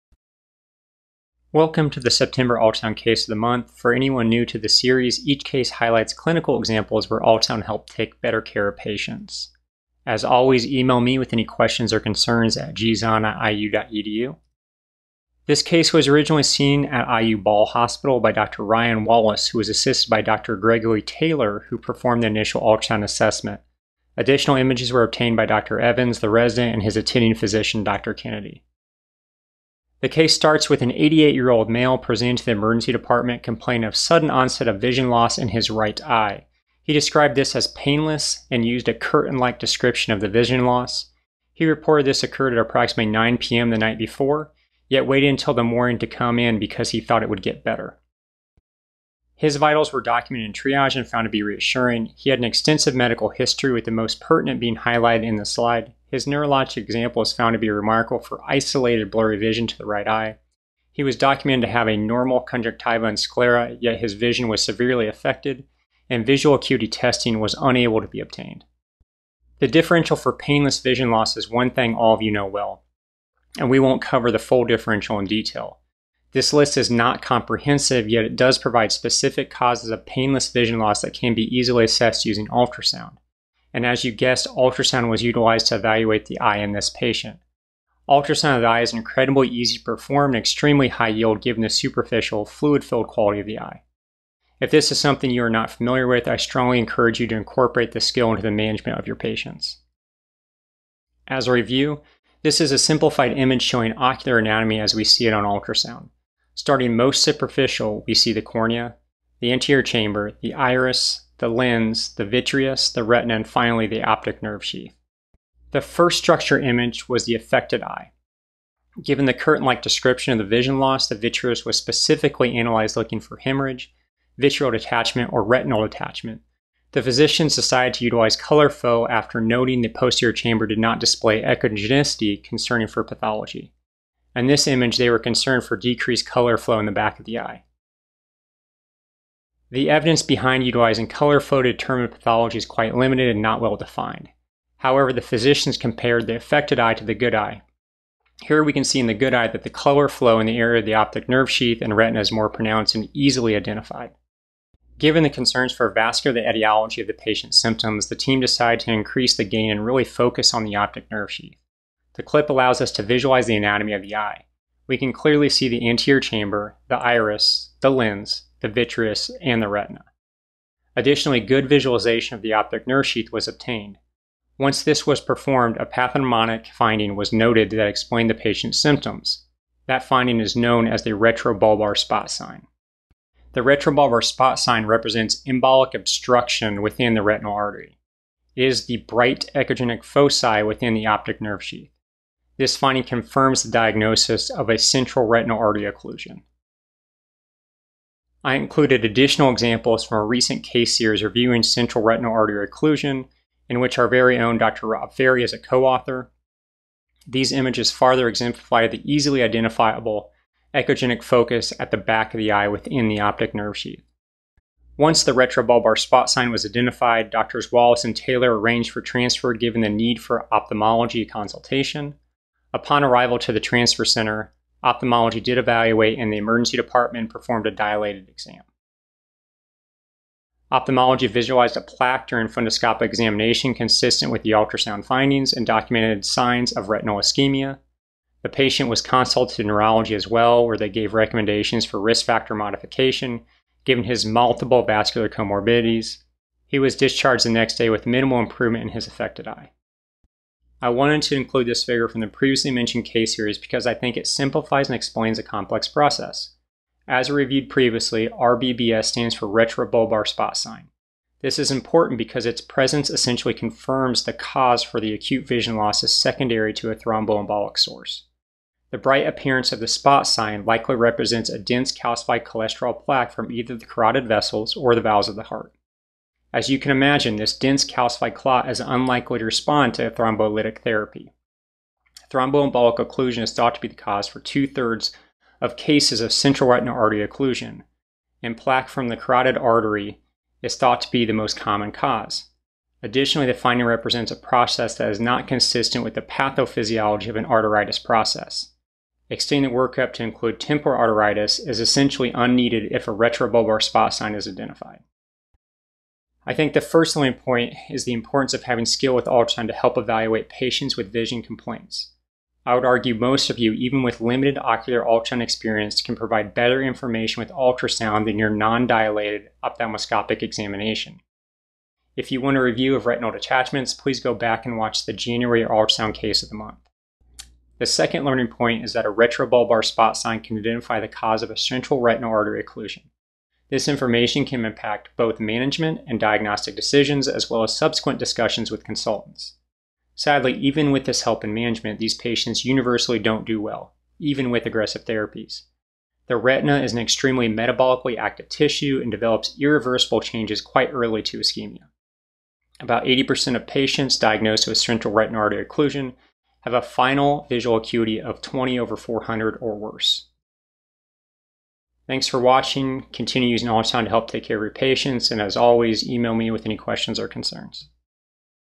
Central Retinal Artery Occlusion Ultrasound: 9/2020
Clinical case discussing a patient with sudden onset vision loss, utilization of ultrasound to identify a central retinal artery occlusion by visualization of the retrobulbar spot sign (RBSS).